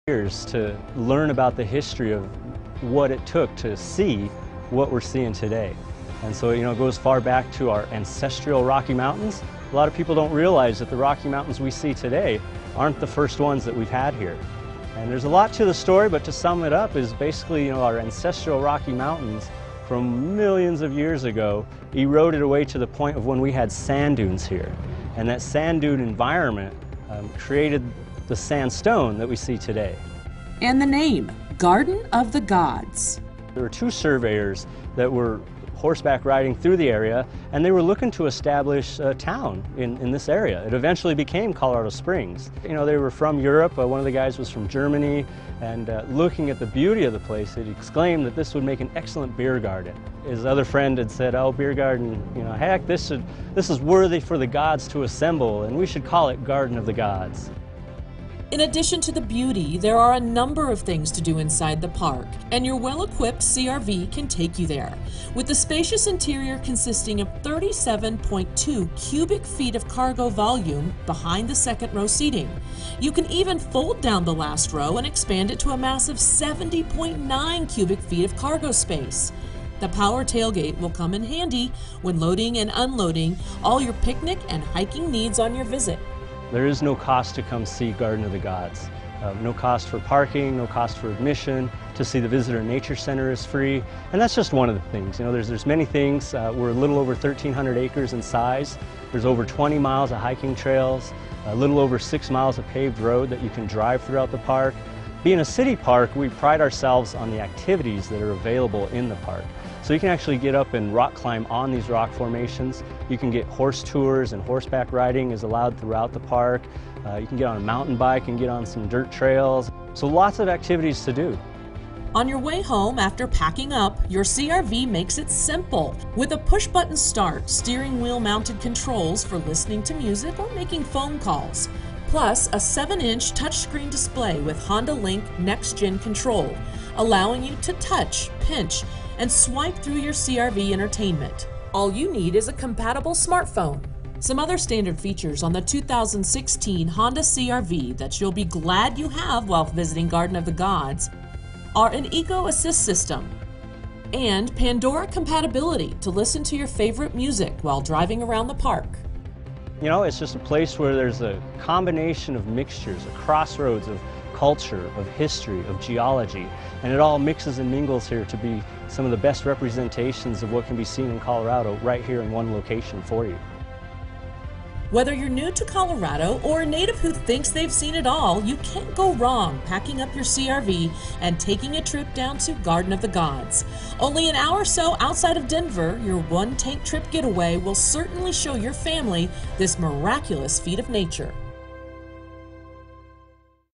Garden of the Gods Discover Colorado
Garden of the Gods is a stunning public park located near Colorado Springs.